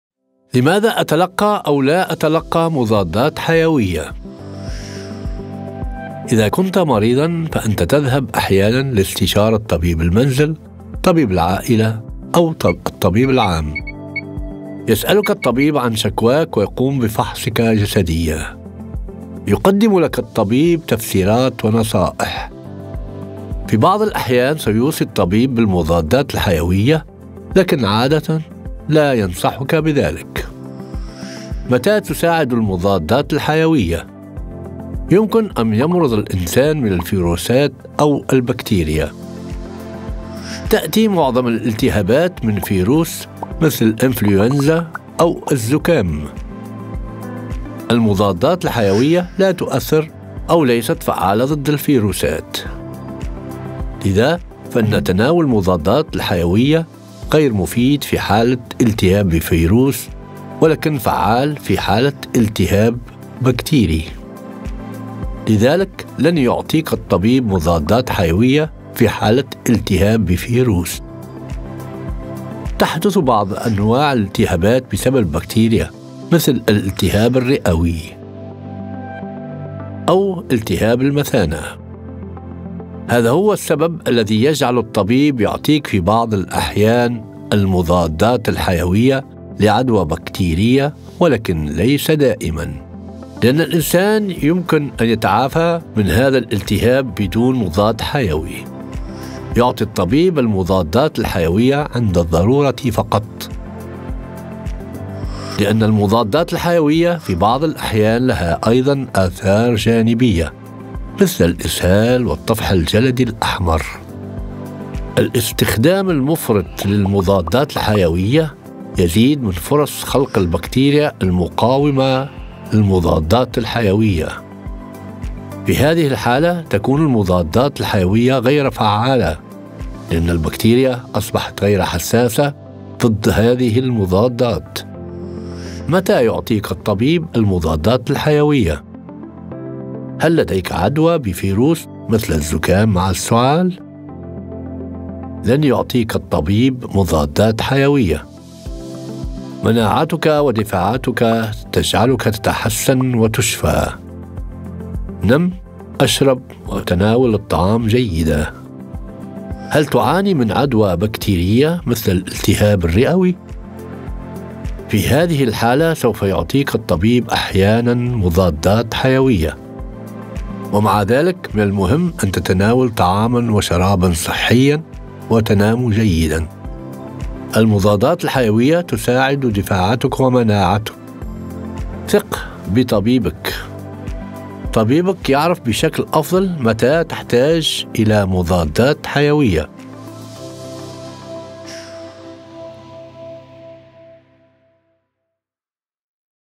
Arabisch - Waarom krijg ik wel of geen antibiotica?
Animatie ter ondersteuning van cultuur-sensitief communiceren met migranten met een focus op de communicatie rondom antibiotica.